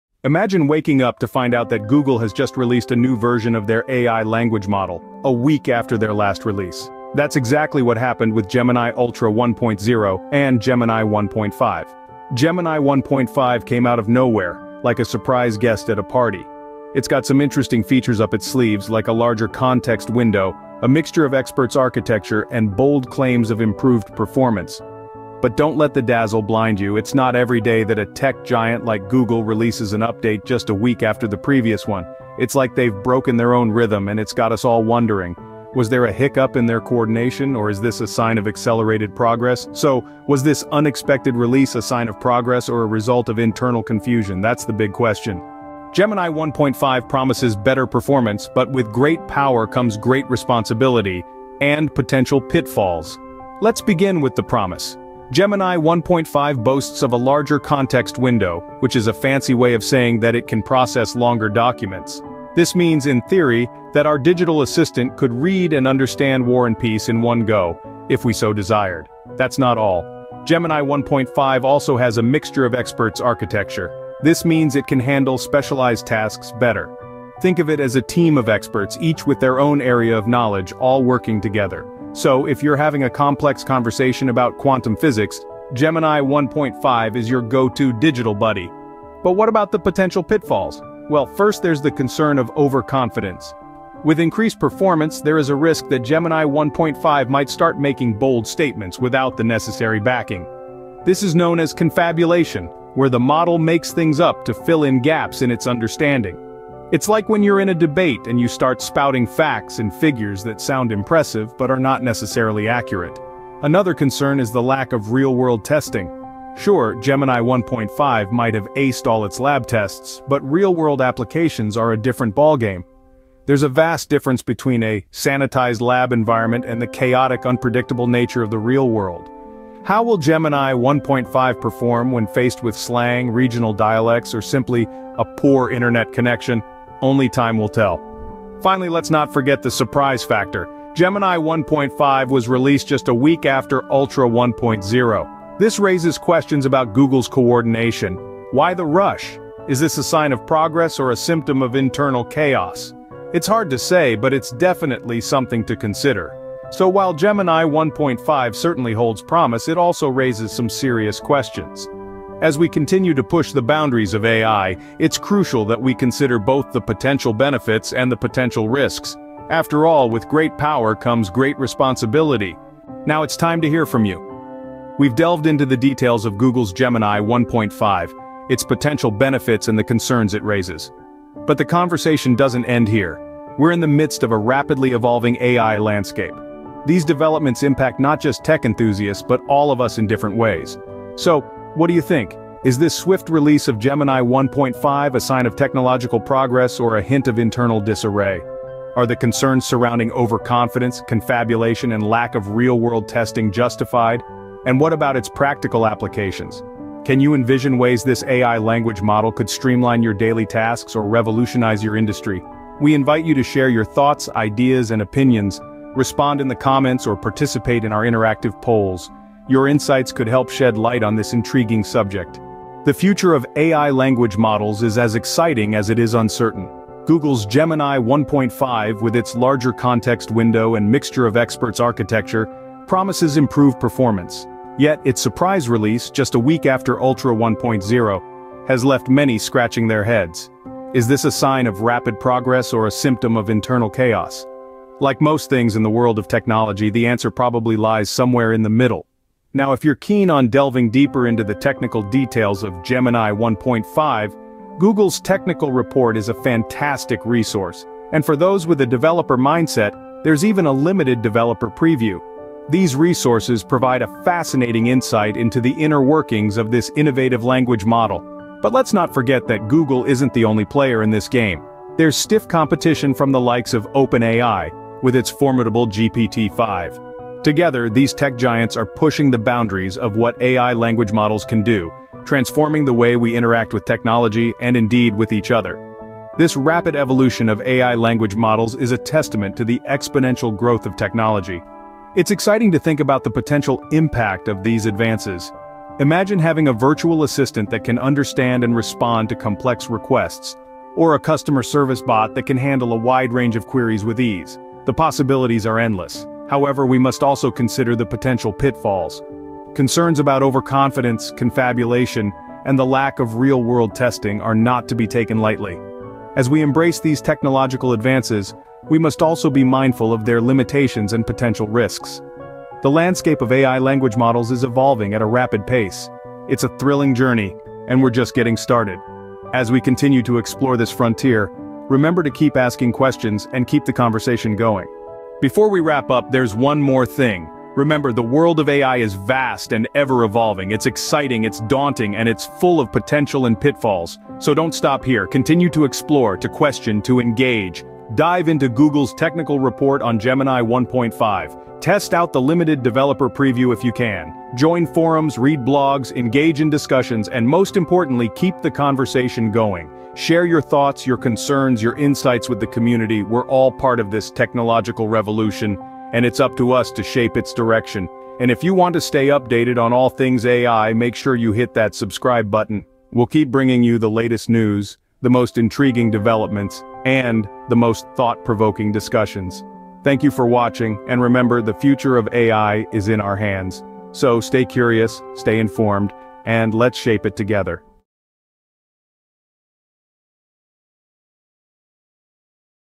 Google's Gemini 1. 5 Breakthrough
Google recently released Gemini 1.5, a new "AI language model" claiming better performance and longer context than its predecessor, Gemini Ultra 1.0.
This announcement surprised many as Ultra 1.0 was just launched a week prior, raising questions about Google's marketing and internal coordination.
Gemini 1.5 boasts a 1 million token context window, significantly larger than previous models, allowing for longer document processing and conversations.
The new model uses a "mixture-of-experts" architecture for specialized tasks and claims impressive reasoning capabilities, but concerns remain about potential inaccuracies and overconfidence.
Google released a technical report and offers a limited preview for developers, but Gemini 1.5 hasn't reached the public chatbot yet.